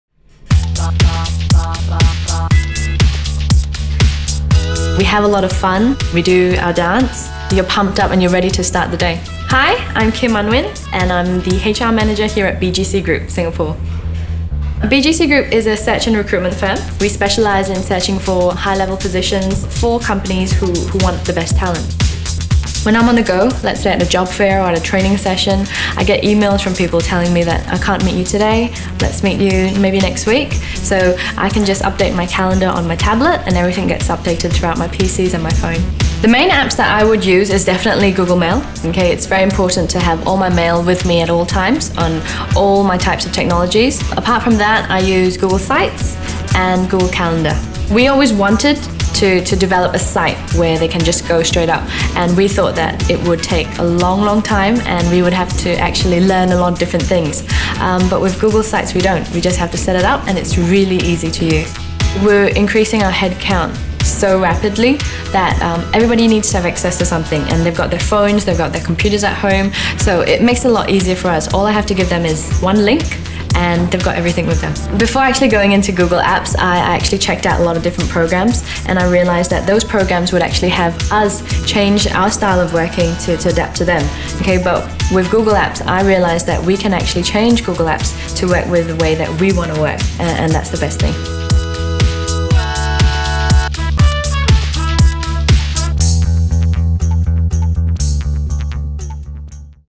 BGC Group has gone Google
As HR Manager for BGC Group, Kimberly is constantly trying to keep others organised. This is why they use Google Apps which helps them not only stay across whats new but also consolidates all the information available to new employees in one central Google Site. For Kimberly it's also about being able to keep on top of her busy schedule while outside the office and Google Apps helps her do this from any device, anywhere, at anytime.